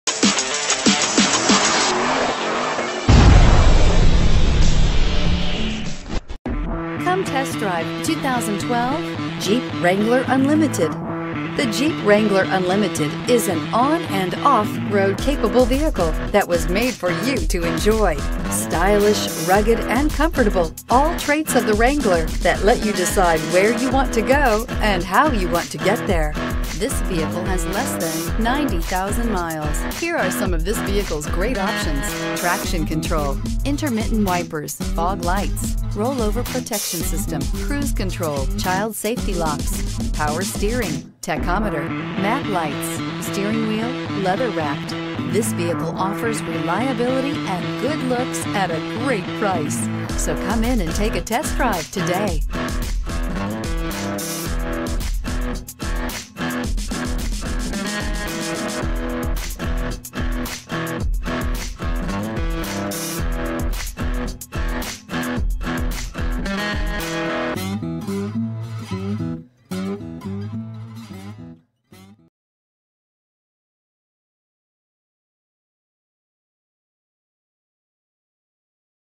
2012 Jeep Wrangler Unlimited Sport Rogers, Blaine, Minneapolis, St Paul, MN 8794
2012 Jeep Wrangler Unlimited Sport

Bedrock Motors
13830 Northdale Blvd.

2 OWNER CLEAN CARFAX !! LIFTED !! TIRES ARE NEAR NEW !! SUPER CLEAN . NICE BLACK WHEELS!! HARD TOP !!